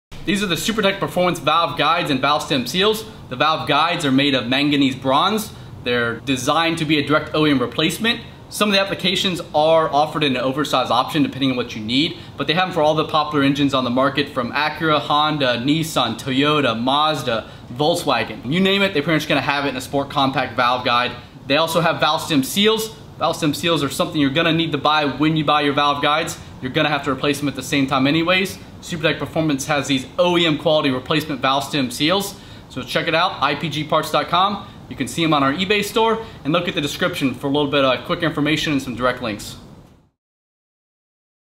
Supertech Performance Manganese Bronze Valve Guides & Valve Stem Seals
Supertech Performance Valve Guides feature precision machining from high quality manganese bronze material. These guides in combination with our low friction black nitrided valves provides the best solution for performance and durability.

Supertech Performance viton valve stem seals are reduced outer diameter valve seals, Perfect for those that are using dual valve springs that may otherwise have valve spring clearance issues. Also a performance option for those that just need to install new valve stem seals due to worn pieces.

- Ebay store